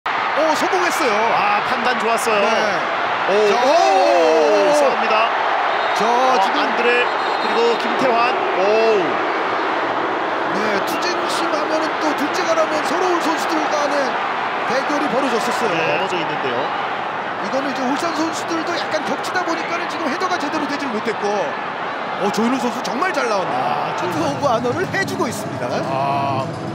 아마노 준 덕분에 화목한 K리그
아마노 준 덕분에 화목하게 몸싸움을 하는 K리그